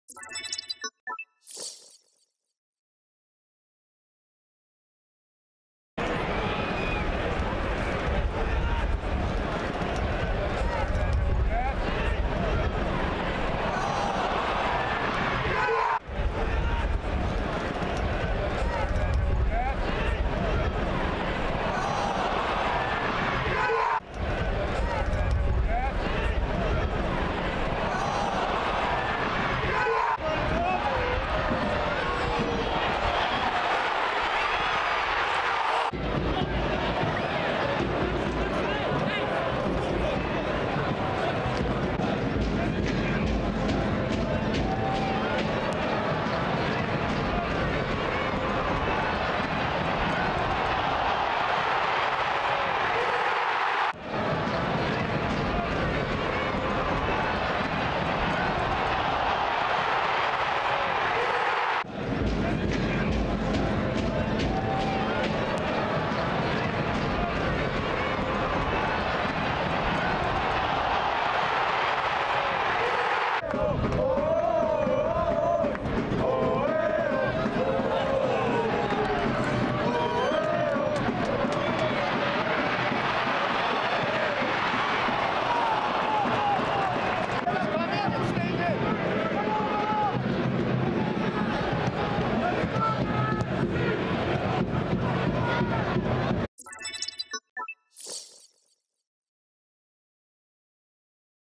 HIGHLIGHTS: Forge FC vs York9 FC [Game 1, April 27]
Full game match highlights of the inaugural game of the Canadian Premier League between Forge FC vs York9. Final score ended in a 1 - 1 draw, boding well for a hot 905 Derby rivalry!

Played at Tim Hortons Field in Hamilton, Ontario, this game was a historic moment in Canadian soccer history and a definitive look at the future of the game in Canada.

- Goal scorers: Ryan Telfer (York 9) & Kadell Thomas (Forge FC)
- Man of the match: Nathan Ingham, GK (York9)

Watch every live game of the Canadian Premier League, including hours of exclusive Canadian soccer content